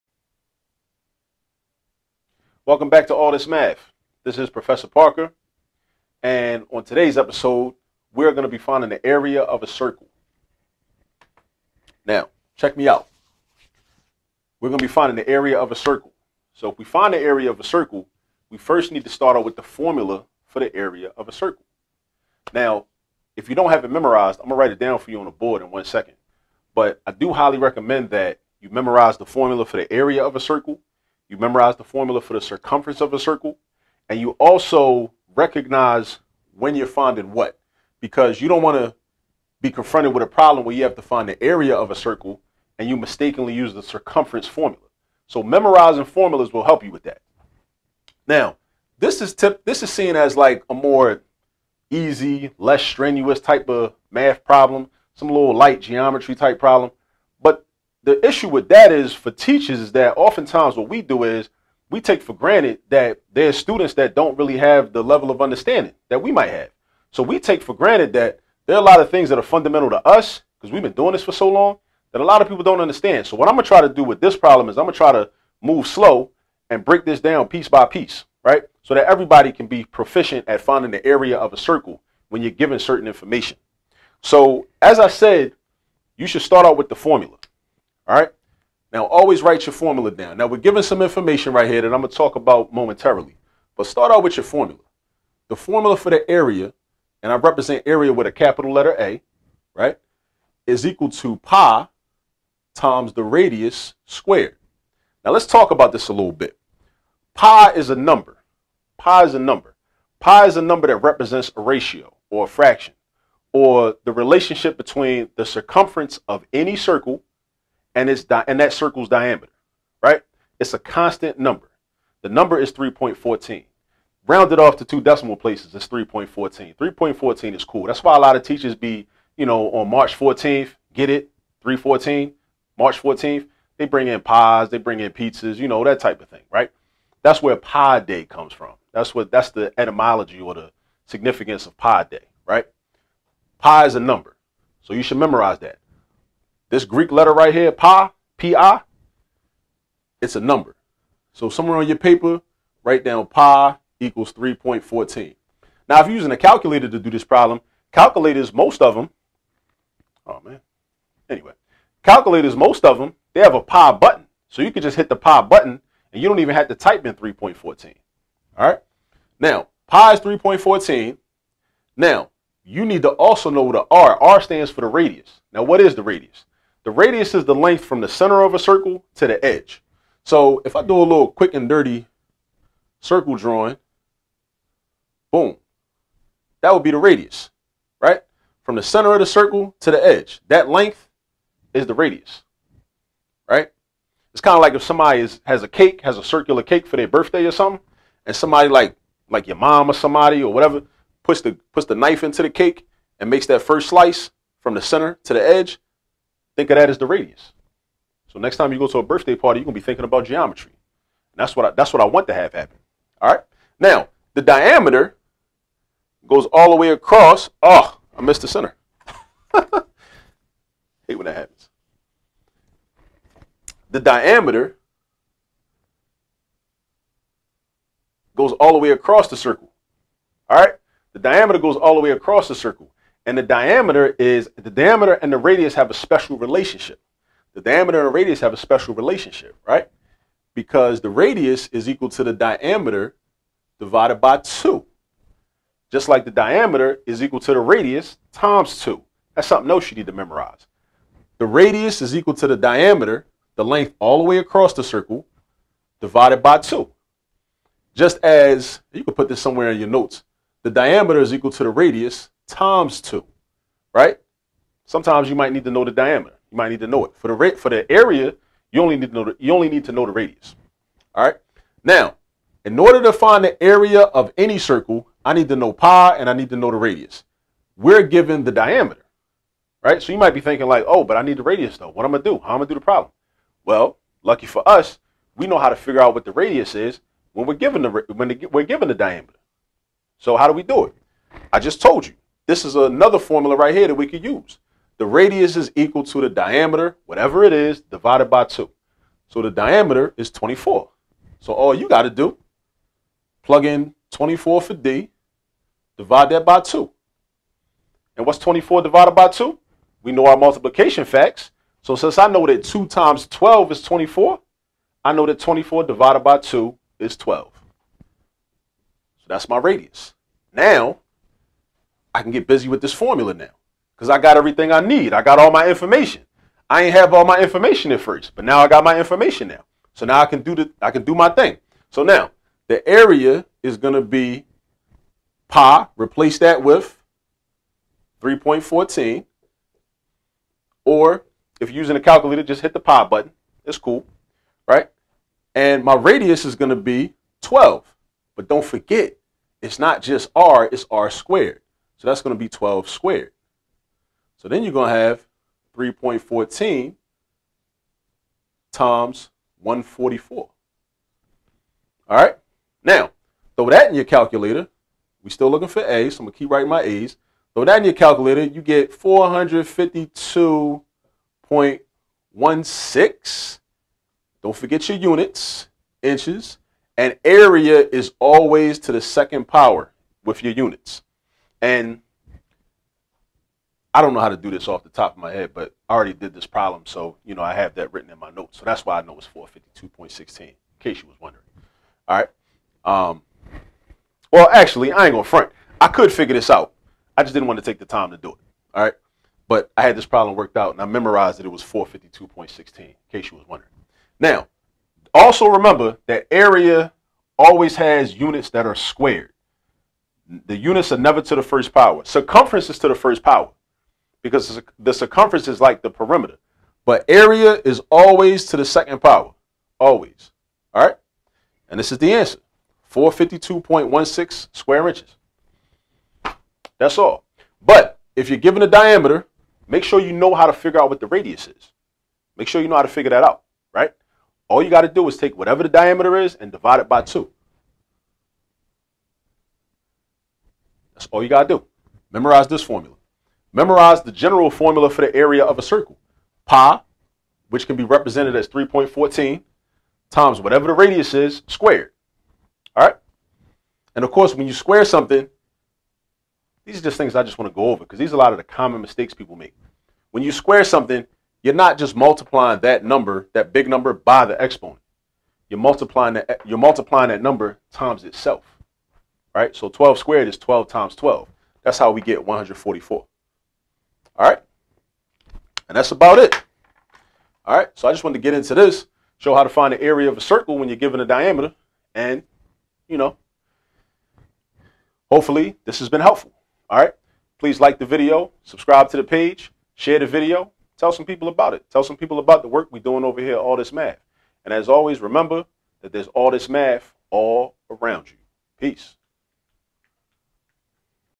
Find the Area of a Circle@AllThisMath
Professor Parker shows how to find the area of a circle when given only the diameter of the circle. Enjoy.